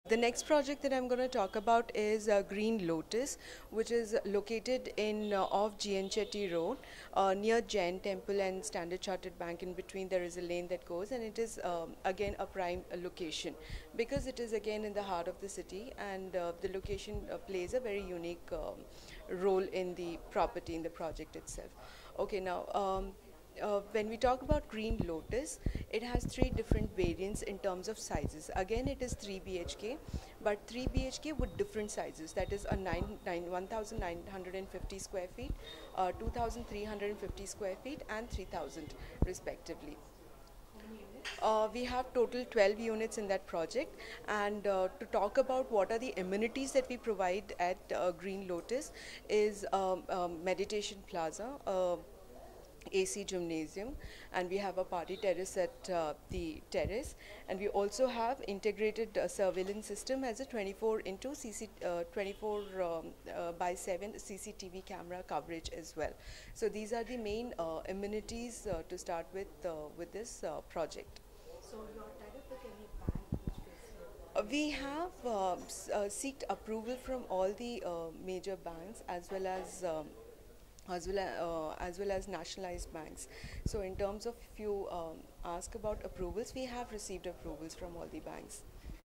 GVSPL Interview Part 2 with Sulekha @ Zak Property Expo 2014
This is a special interview provided by Green Valley Shelters Private Limited about their luxury project during the Zak Luxury homes property expo 2014 organized from 7 – 17 August 2014 at Zak Expo & Convention Centre, Alwarpet. The second part of the interview showcases their Green Lotus Project in Chennai.